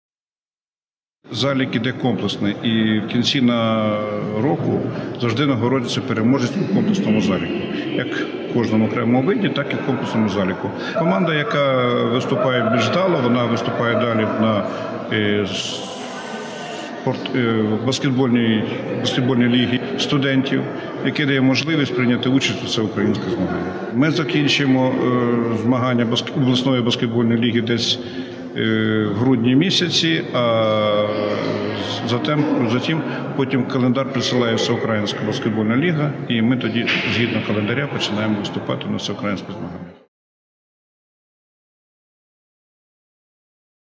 Стартувала Хмельницька міська спартакіада з баскетболу серед студентів вузів 3-4 рівнів акредитації
Стартувала міська спартакіада з баскетболу серед команд 3-4-го рівнів акредитації. Грають 4 команди-повідомляє головний суддя змагань Сергій ЛозІнський. У першій зустрічі змагань перемогла команда Хмельницького університету управління та права з рахунком 52:43. Матч поміж курсантами Національної академії державної прикордонної служби з командою хмельницького Національного університету закінчилася перемогою прикордонників 42:18.
Розпочали змагання команди Хмельницького університету управління і права-вони грають у бузковій формі, та Хмельницького кооперативного торговельно-економічного інституту-вони в синій формі. На третій хвилині студенти ХУУПу заробили 3 перші очки, ще за дві хвилини суперники також влучили в кошик, і протягом першої частини зустрічі команди йшли майже на рівних-каже Сергій Лозінський.